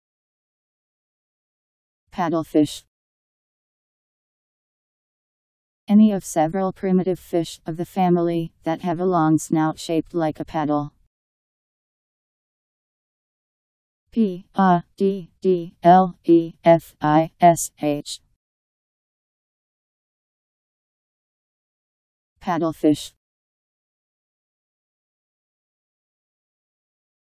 Paddlefish Meaning
Video shows what paddlefish means. Any of several primitive fish, of the family , that have a long snout shaped like a paddle..  Paddlefish Meaning. How to pronounce, definition audio dictionary. How to say paddlefish. Powered by MaryTTS, Wiktionary